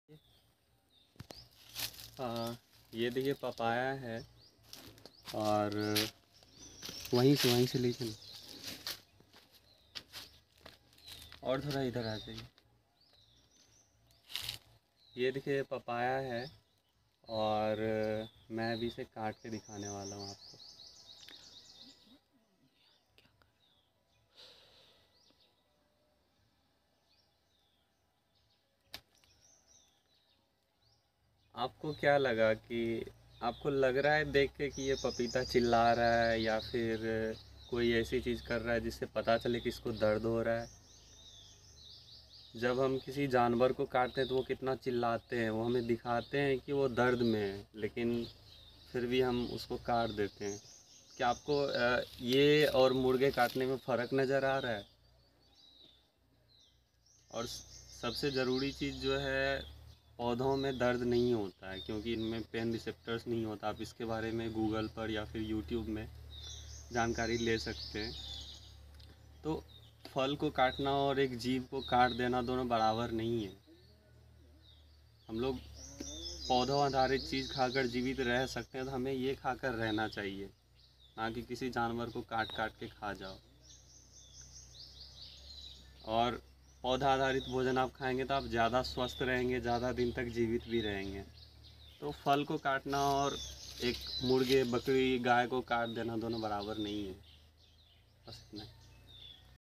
Plant feel pain ? NO
They do not have nervous system like human and animals.